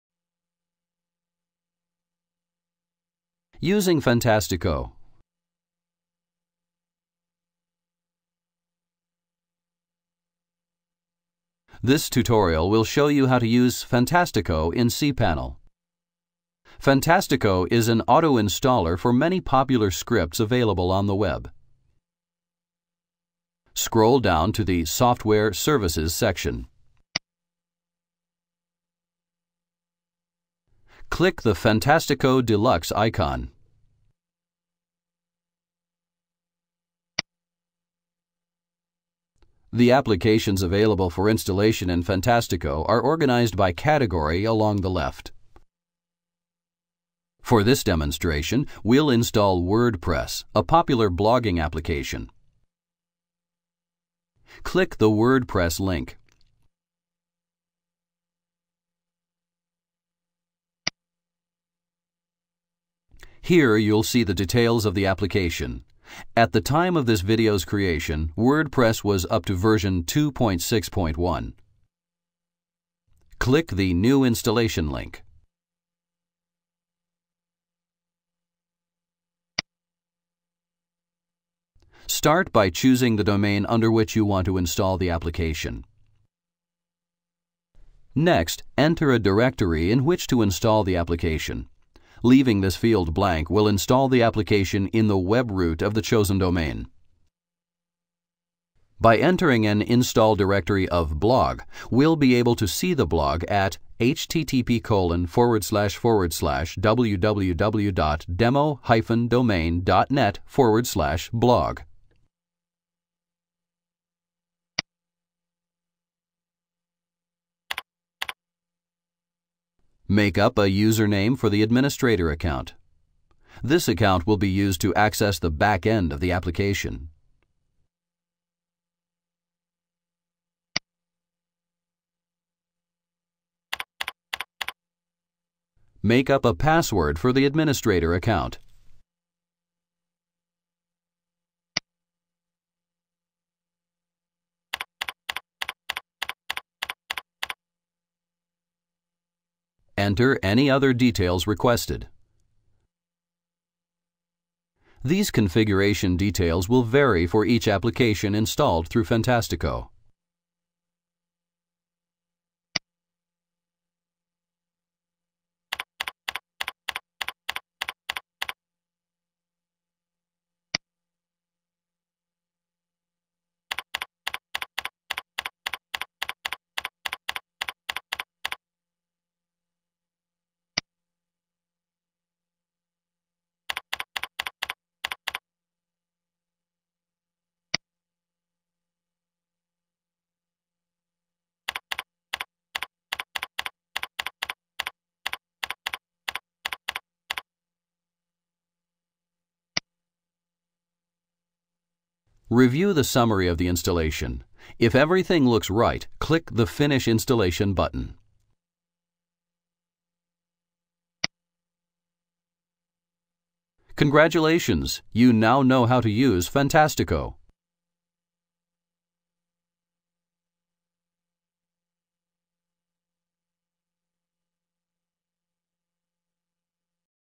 How to use Fantastico
Fantastico allows you to install some of the most popular opensource software in just a few click. Programs like Wordpress, Joomla, Phpbb, and various others can be automatically installed for you by filling out a simple form, Fantastico does all the hard work for you.